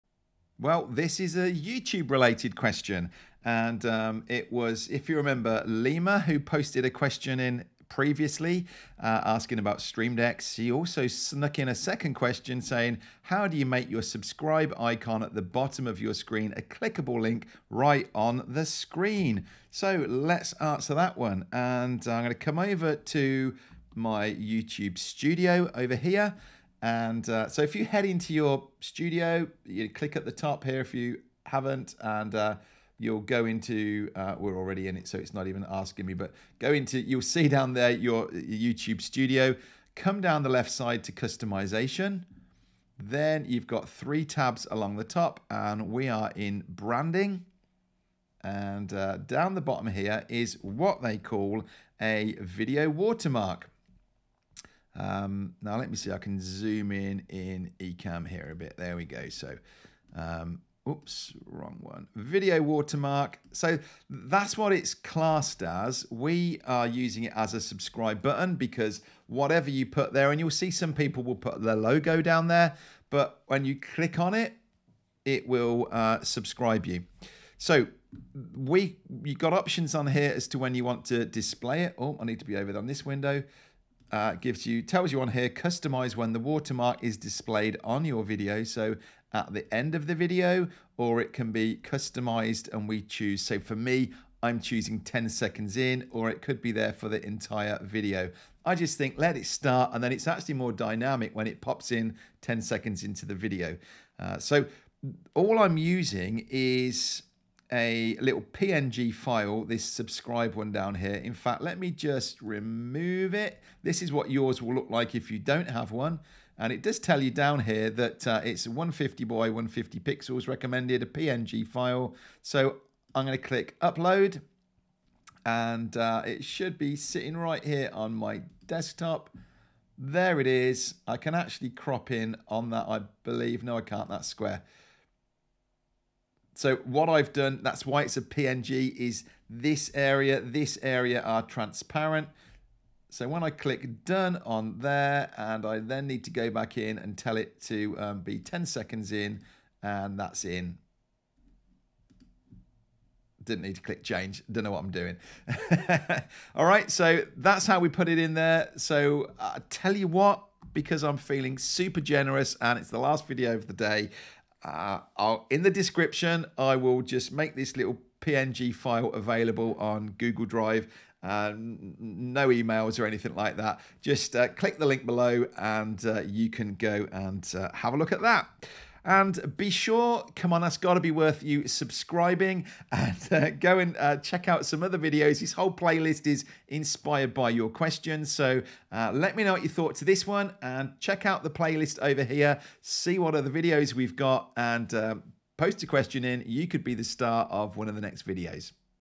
How To Add A Clickable Subscribe Button To YouTube Videos
Do it once and it applies to ALL your videos, past and future!